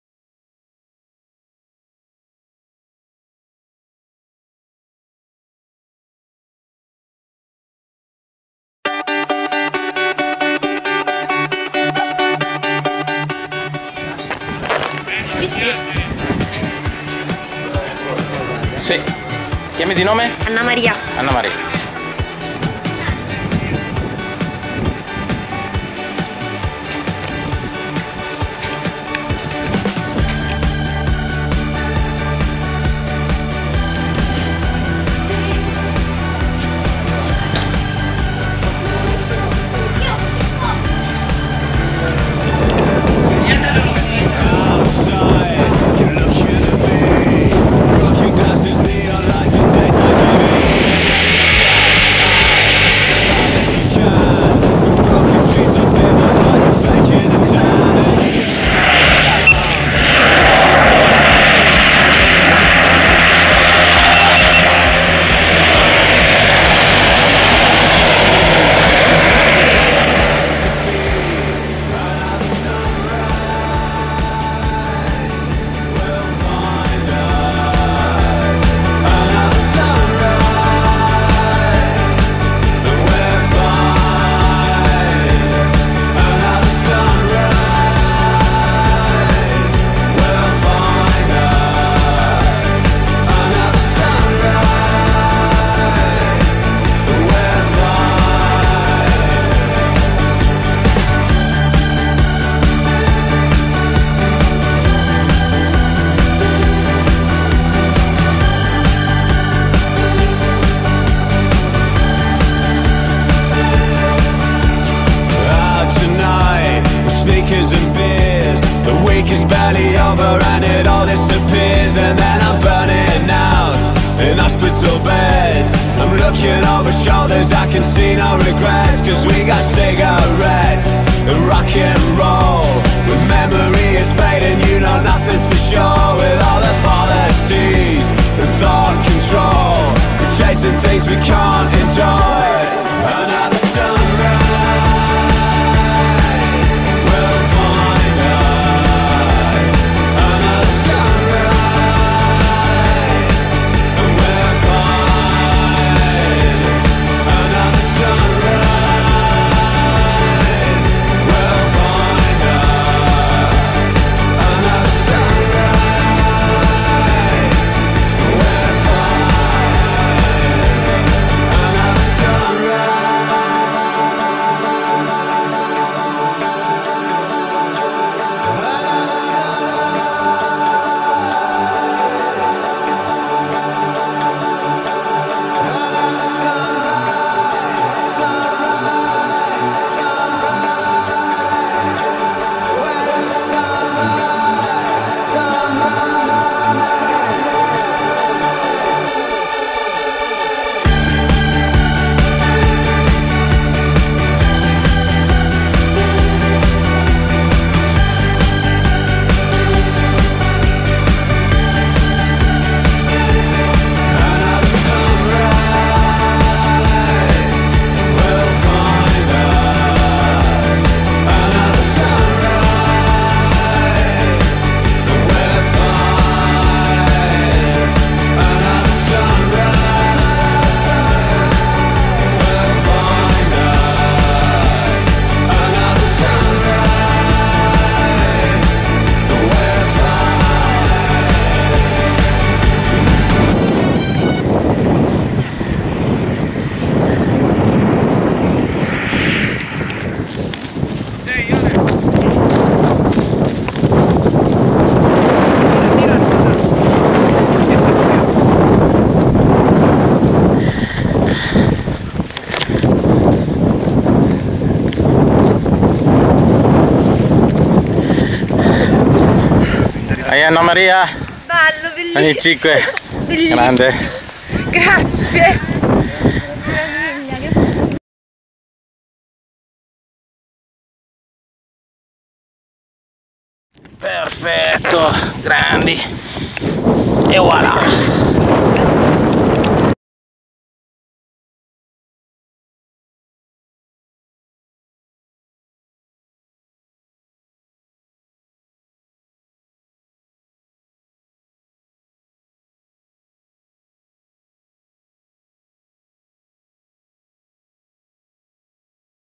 Tandem Icake Annamaria
Salto in tandem con istrutture di paracadutismo a Casale Monferrato